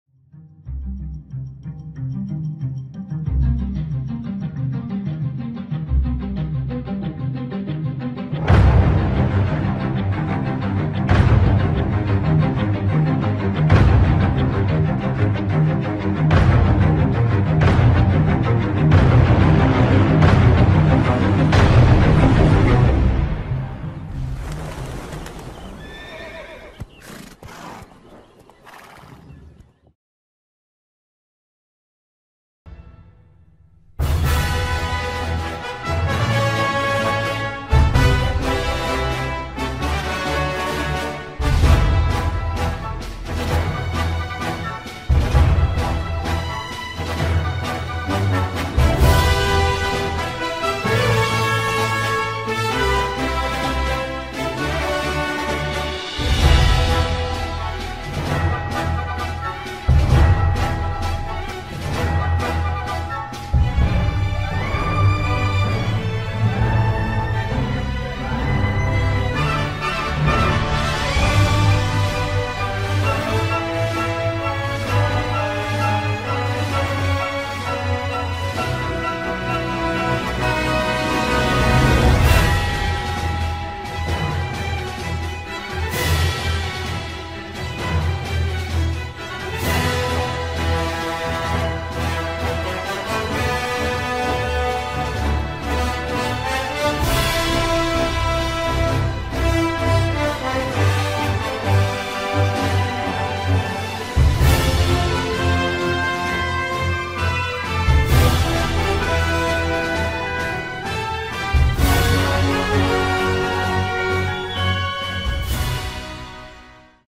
Trailer de la chaîne
Trailer réalisé par moi-même que je trouve assez épique et qui résume bien mon style de jeu et mes projets sur la chaîne.